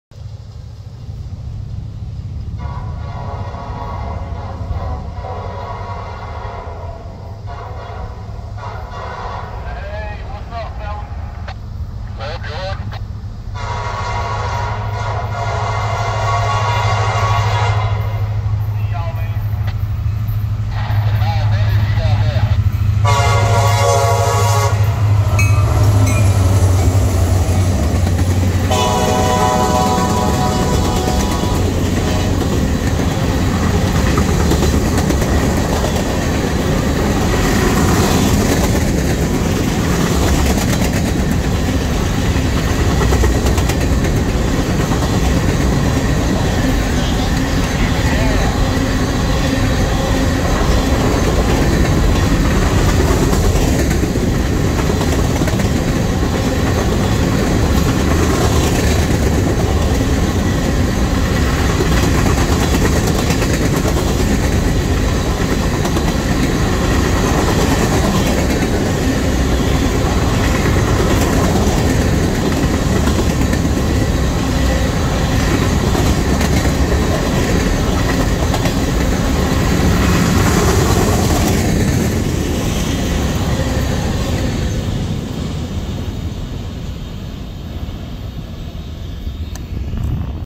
CSX South End Sub: CSX G960-08 w/ SD70MAC, SD70MACe! (Benson, NC 7/12/22)
I'm speechless. MAC duos are already stupid rare around here, and 2 of them passed within 2 days! I'm certainly not complaining though!

CSX G960-08
Power:
CSXT 4758 - SD70MAC
CSXT 4552 - SD70MACe